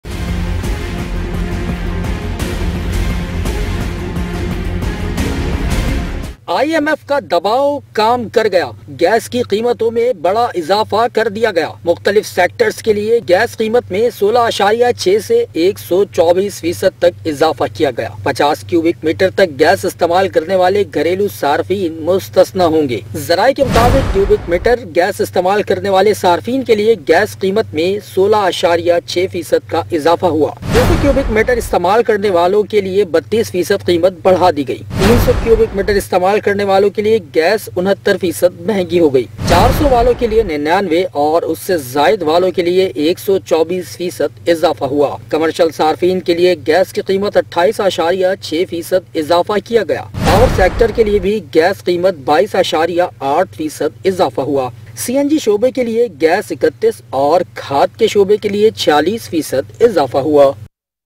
Gas prices were hiked heavily due to strict IMF conditions - Aaj Markets
Gas prices were hiked heavily due to strict IMF conditions -  14 Feb 2023 - Aaj News

Aaj TV Official:
shorturl.at/iBDNX